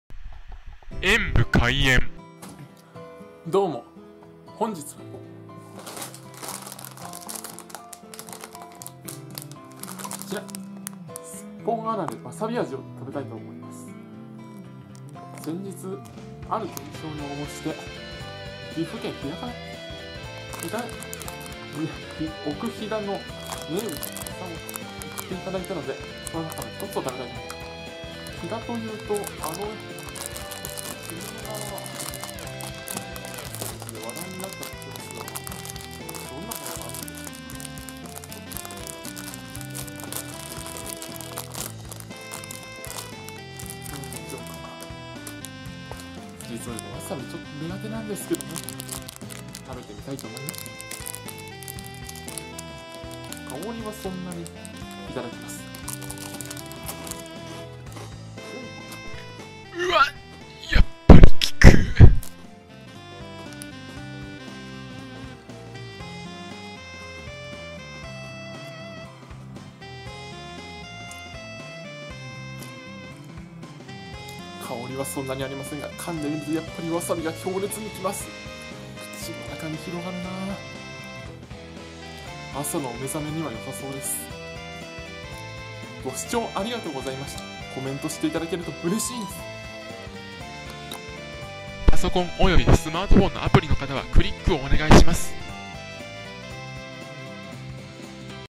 話題になったあの地から 奥飛騨の山葵あられを食べてみたら…
奥飛騨の山葵あられを食べてみたのですが…
とんでもない事に…